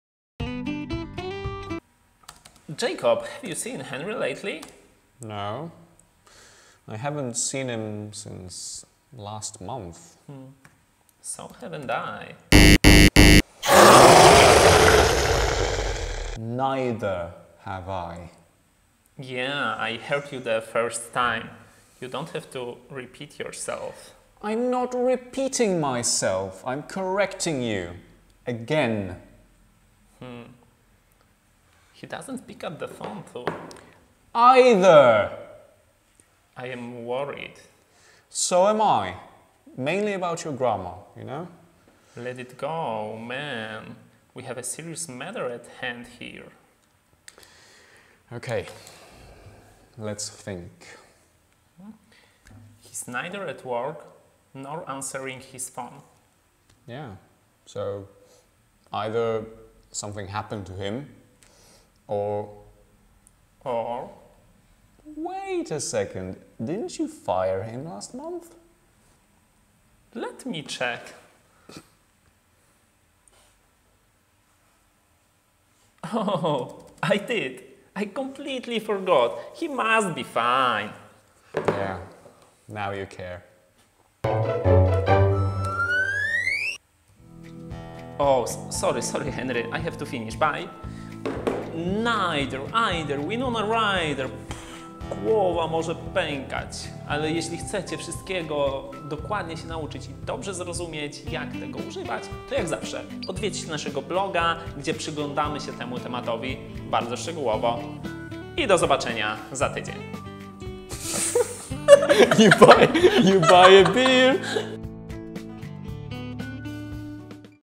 EITHER czy NEITHER?
EITHER czy NEITHER? Co te słowa w ogóle znaczą? I jak to się wymawia? Jeśli zadajecie sobie te pytania, to koniecznie obejrzyjcie poniższy materiał wideo i przeczytajcie nasz artykuł na blogu:
Na blogu przyjrzymy się również najbardziej powszechnym strukturom gramatycznym, jeśli chodzi o EITHER i NEITHER.

Możesz również sprawdzić nasze inne kanały i sięgnąć po więcej materiałów do nauki 🇬🇧 języka angielskiego!

Do zobaczenia w następną niedzielę!